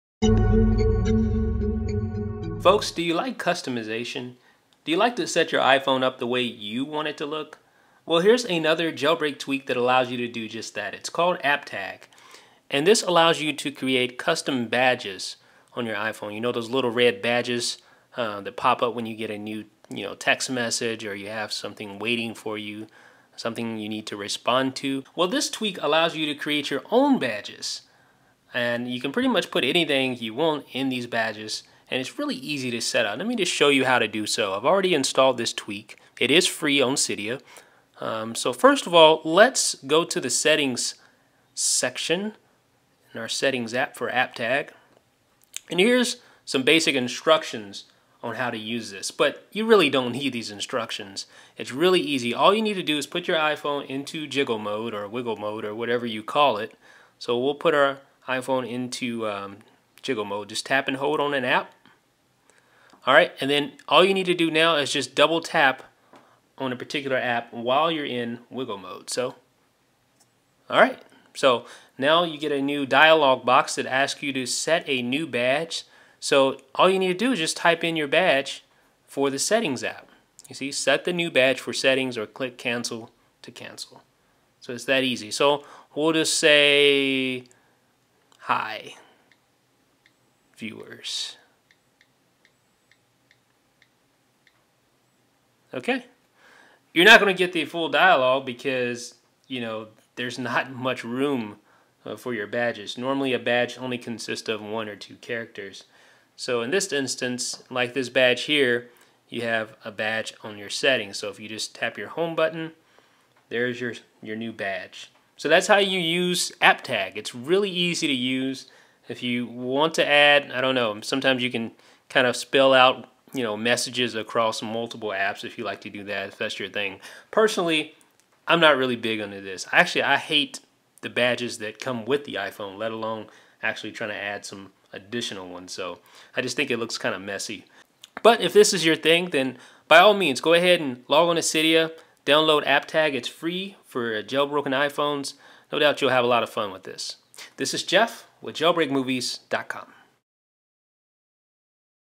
'AppTag' Adds Custom Badges to Your iPhone's Apps
Want to add custom badges to your iPhone's apps? There's no shortage of ways to do so, but the latest way is AppTag, a free jailbreak tweak available for the iPhone in Cydia.

About iDB